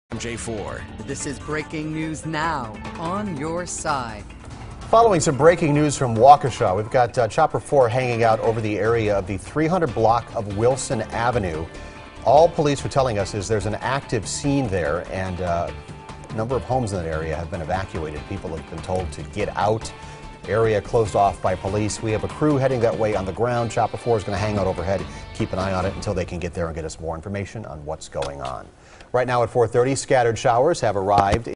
Waukesha residents being evacuated
Police are evacuating residents in Waukesha around the area of the 300 block of Wilson Avenue.